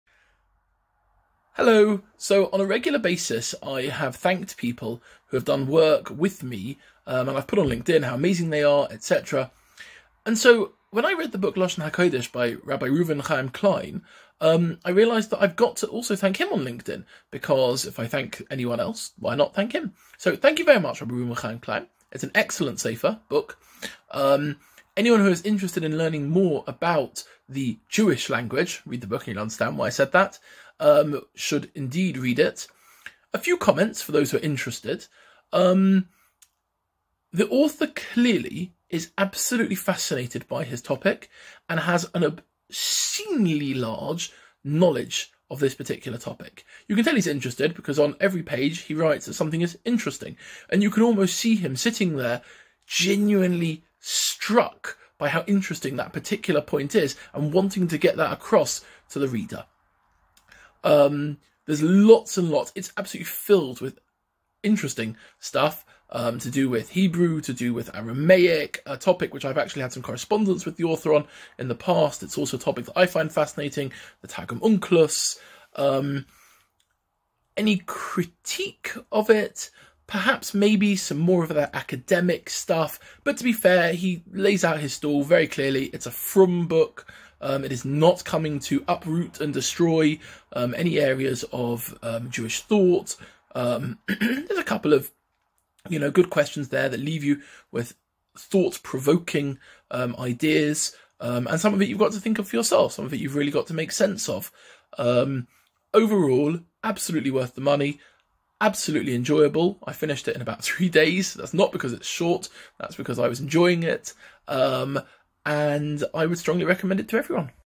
THANK YOU RC Klein
Benjamin Goldberg offers words of praise for Rabbi Reuven Chaim Klein's book Lashon HaKodesh: History, Holiness, & Hebrew (Mosaica Press, 2021).